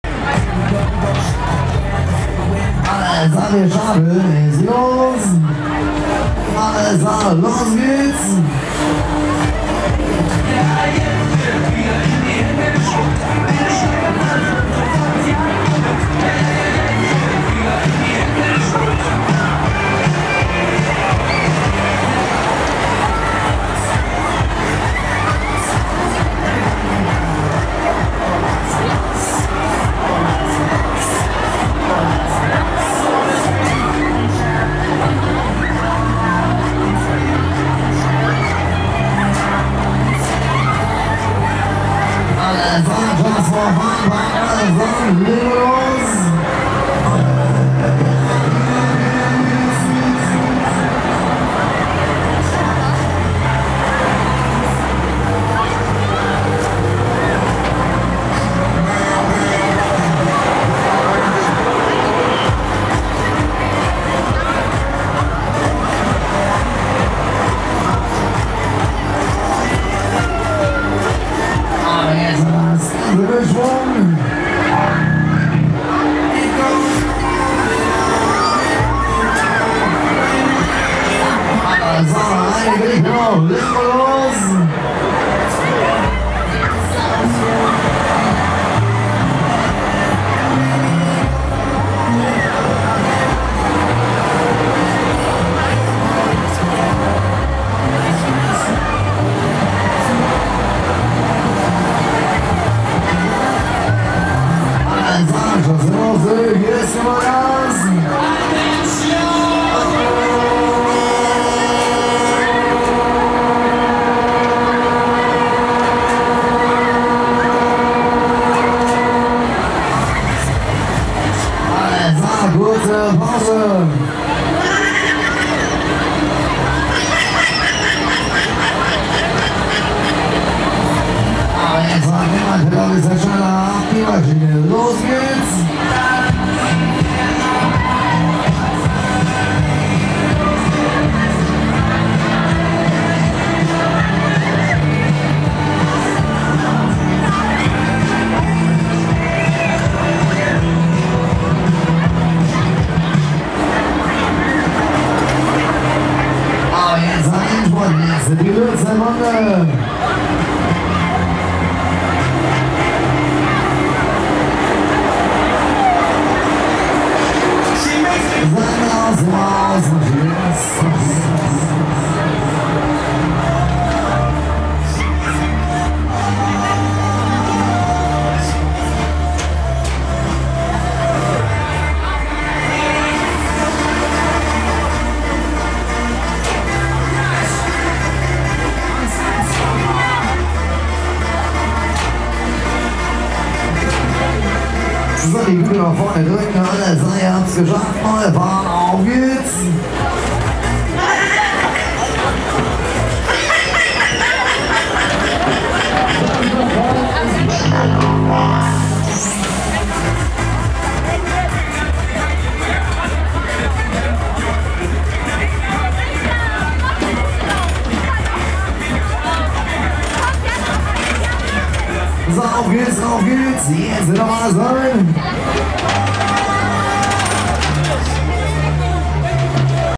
Eclipse - Störzer (Offride) Video Herbstvolksfest Köln 2013
Eclipse - Störzer (Offride) Video vom Herbstvolksfest am Rheinufer in Köln Deutz 2013. Das Video entstand am Mittwoch (Familientag). Die Eclipse reist heute als Kick Down unter Landwermann.